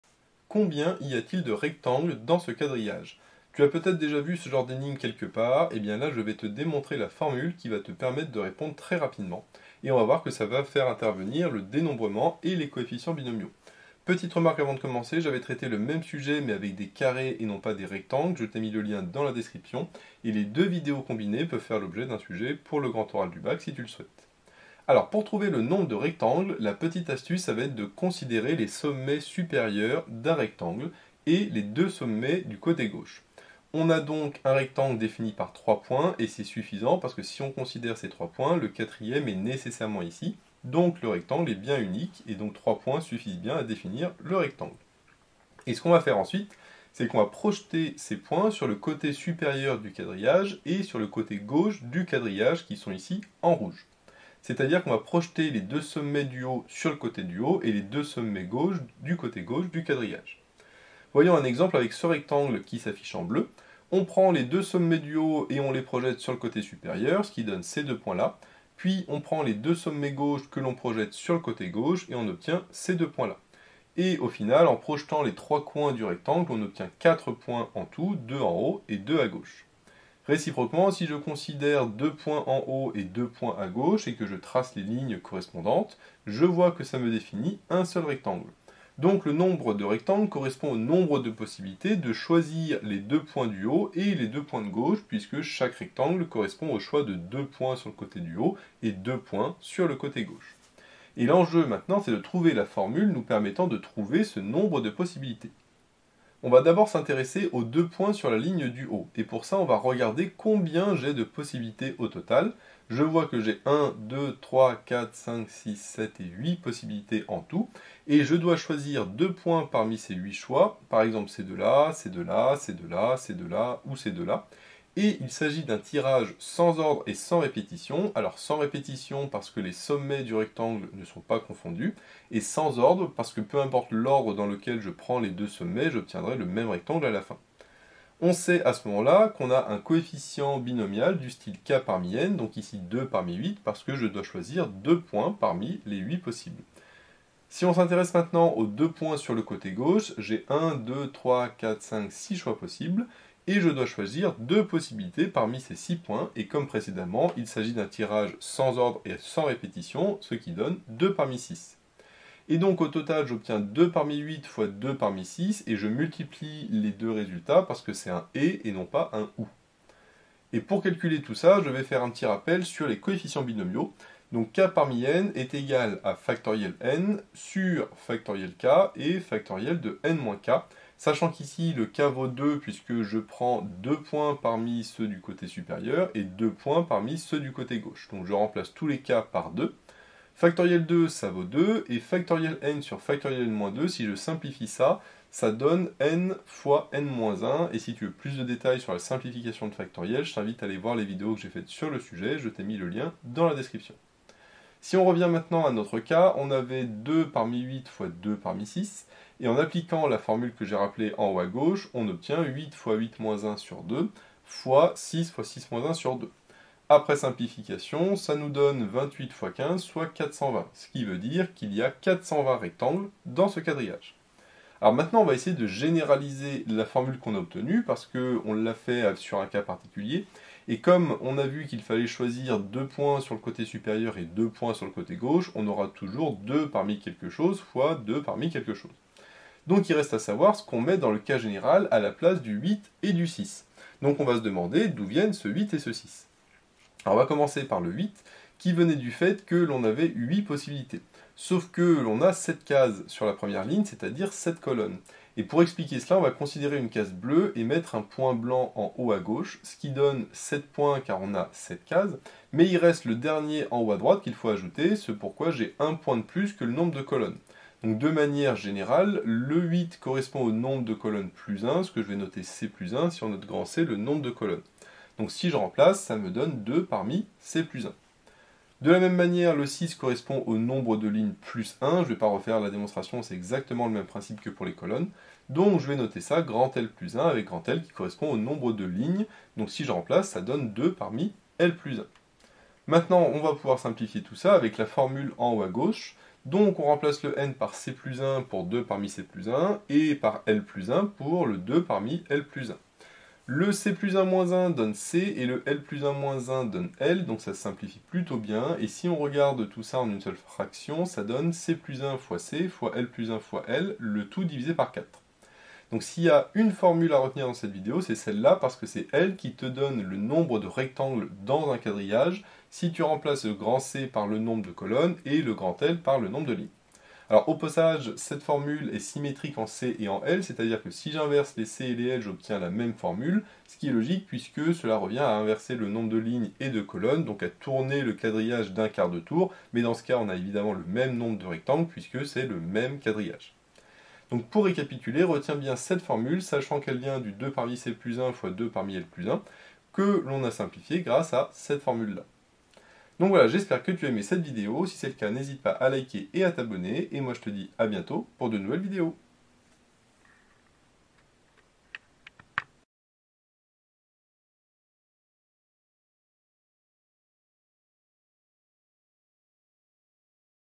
Nombre de rectangles dans un quadrillage - Grand Oral du Bac
Quelle est la formule qui donne le nombre de rectangles dans un quadrillage ?
Pour plus d'infos, des bonus et de nombreux autres exercices corrigés, rendez-vous

00:00 Introduction
00:25 Présentation de la méthode
01:00 Exemple avec un rectangle
01:45 Démonstration de la formule avec le dénombrement
02:50 Rappel sur les coefficients binomiaux
03:25 Application de la formule sur l'exemple
03:45 Généralisation de la formule